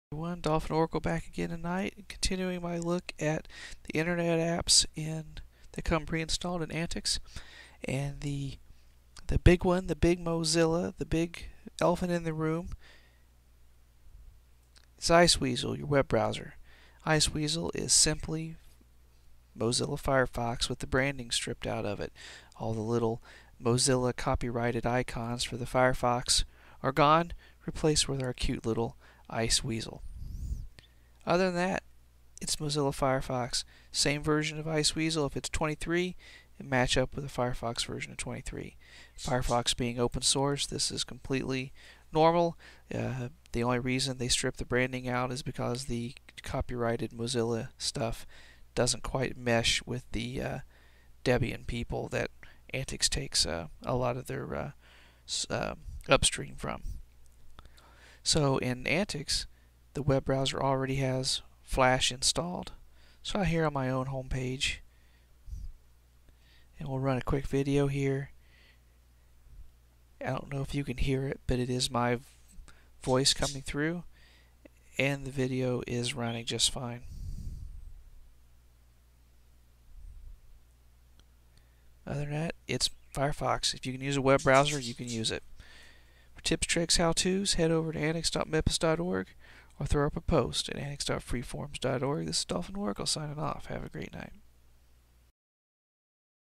AntiX 13 Stable - Iceweasel - Browse the web
A Quick Intro to Iceweasel, the Debian version of Firefox found in AntiX.

Its Firefox, but without the copyrighted branding.

d.o.